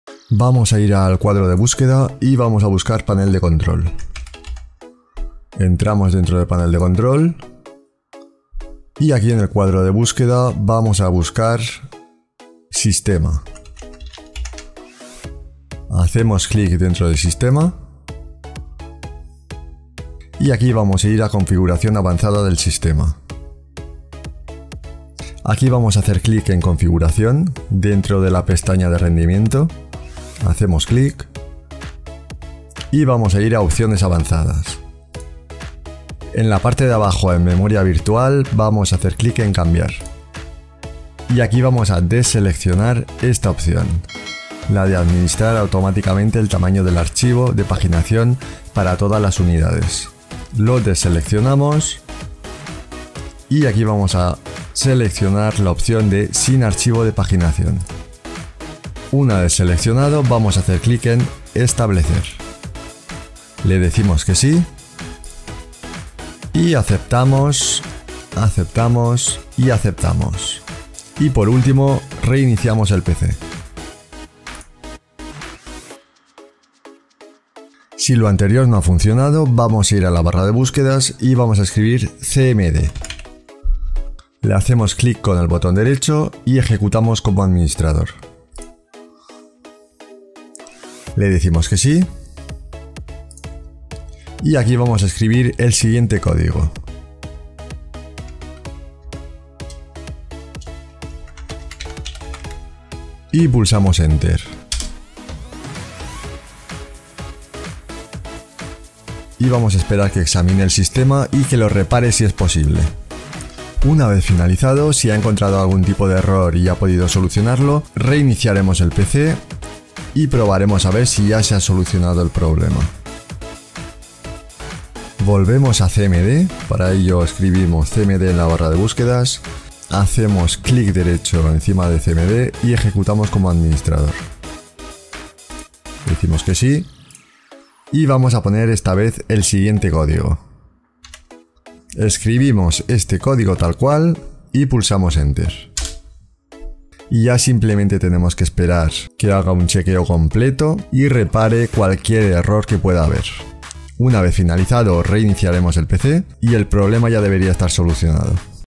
✅ SOLUCION ERROR ISDONE.DLL/UNARC.DLL AL INSTALAR JUEGOS EN WINDOWS 10/8/7
Como solucionar el error ISDONE.DLL y UNARC.DLL al instalar juegos o programas en Windows 10/8/7. Solucion facil para reparar el error de ISDone.dll y Unarc.dll con error code 11, 12, 14, 1 y varios mas al instalar juegos como GTA V, Tomb Raider, Final Fantasy, PES, Far Cry, FIFA y muchos mas.

0:00 | Solución 1
1:16 | Solución 2
2:03 | Solución 3